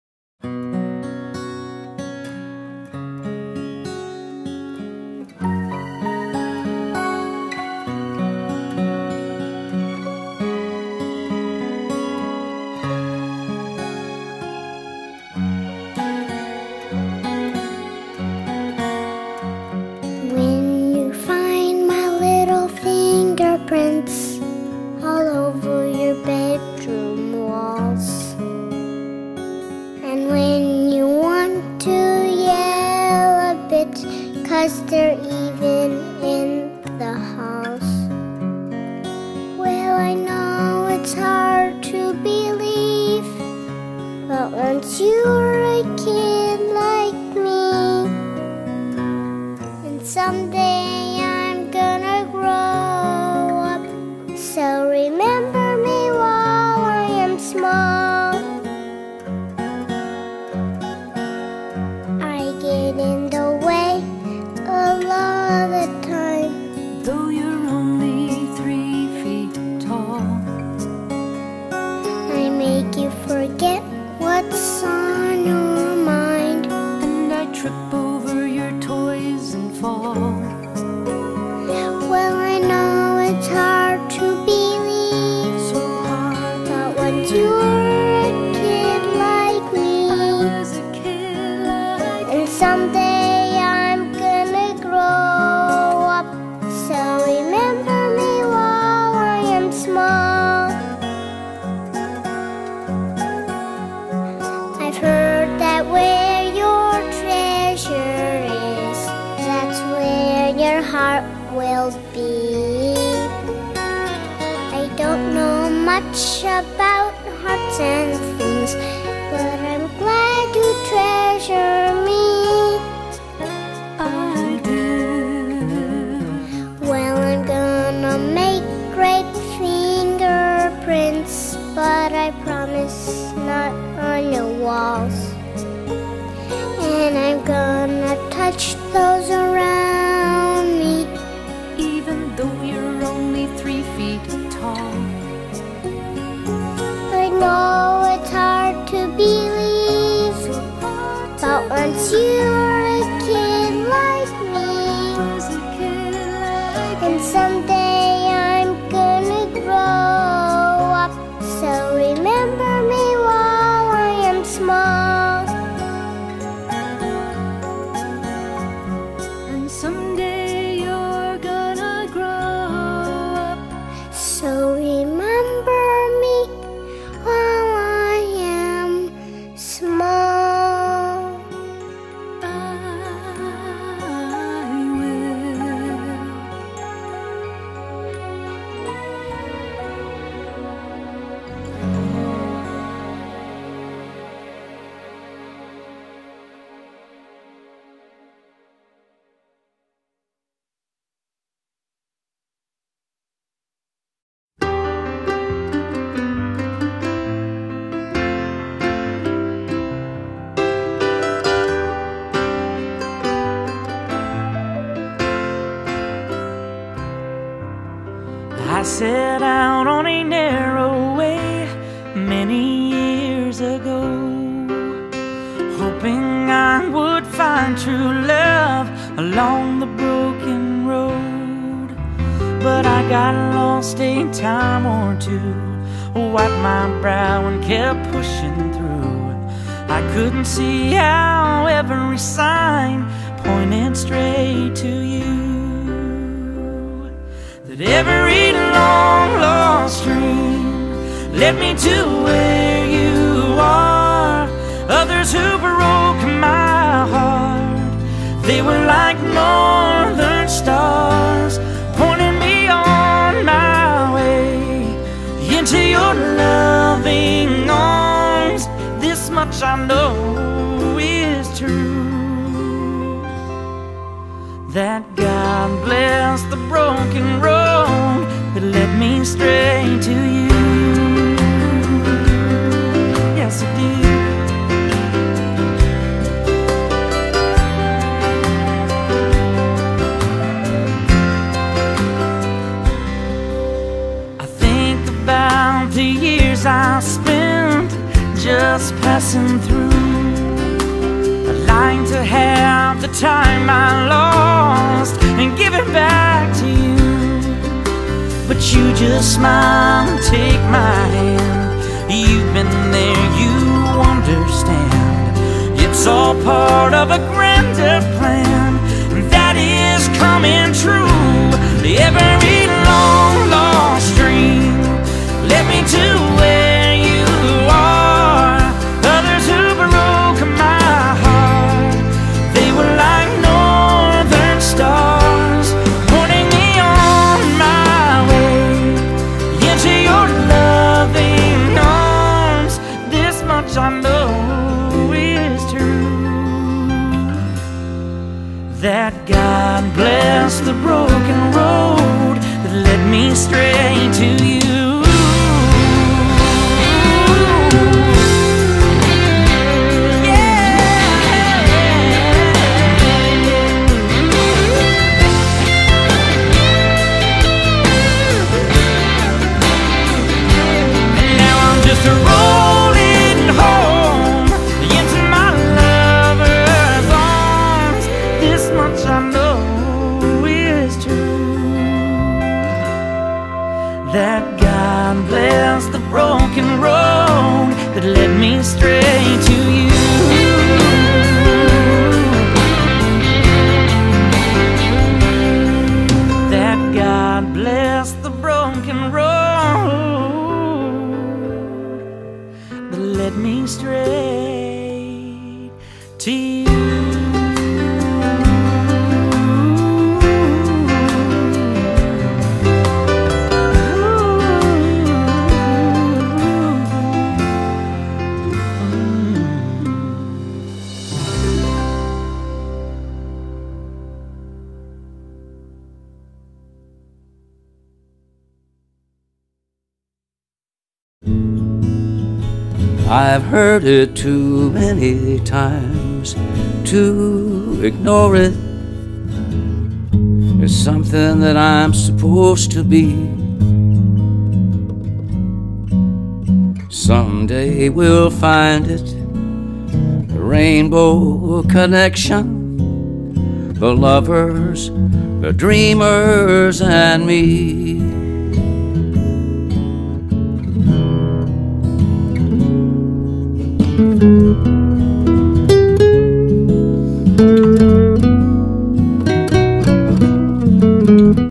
NatalieKaz
Part One of a tribute to my oldest dear daughter who entered our family 10 years ago March 19!